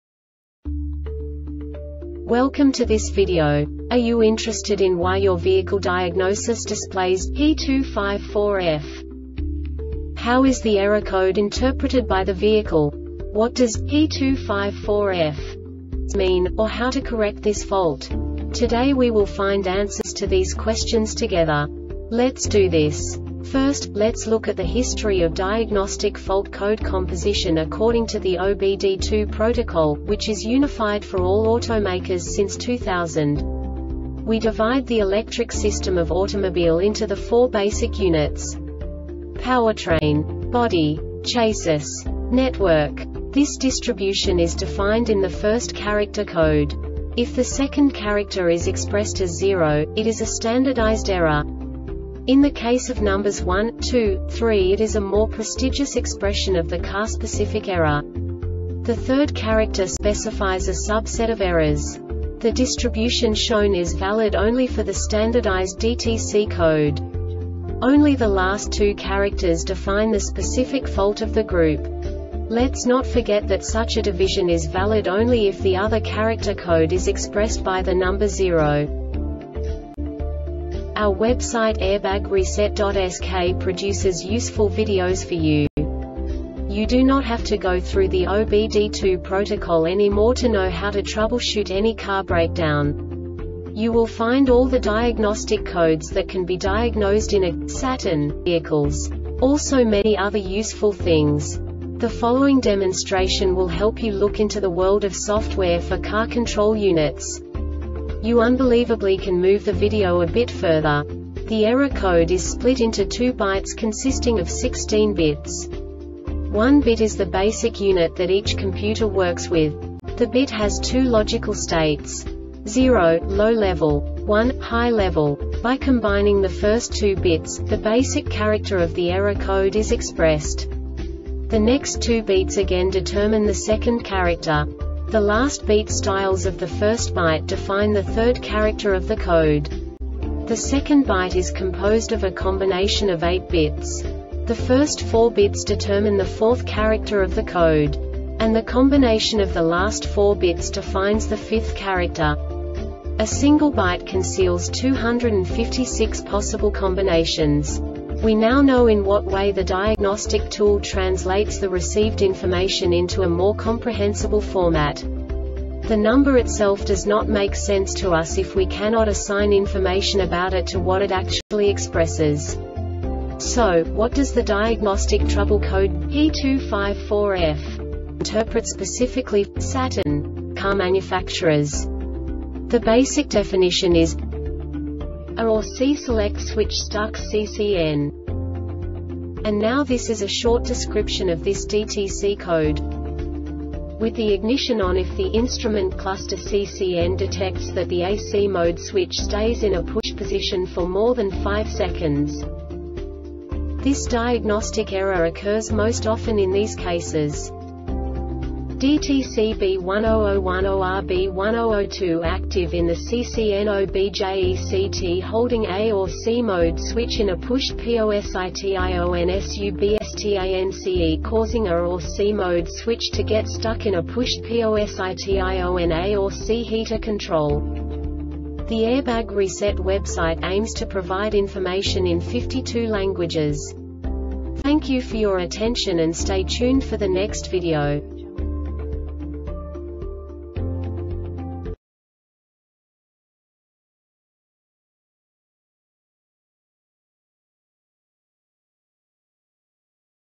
DTC Saturn P254F Short Explanation
Contents:
0:21 Basic DTC analysis according to OBD2 protocol standard.
1:48 Insight into programming
2:58 A diagnostically understandable description of the Diagnostic Trouble Code
3:05 Basic error definition

Make:
Saturn
Code:
P254F
Definition:
A or C SELECT SWITCH STUCK (CCN)
Description:
With the ignition on. If the Instrument Cluster (CCN) detects that the A or C mode switch stays in a pushed position for more than 5 seconds
Cause:
DTC B1001 OR B1002 ACTIVE IN THE CCNOBJECT HOLDING A or C MODE SWITCH IN A PUSHED POSITIONSUBSTANCE CAUSING A or C MODE SWITCH TO GET STUCK IN A PUSHED POSITIONA or C HEATER CONTROL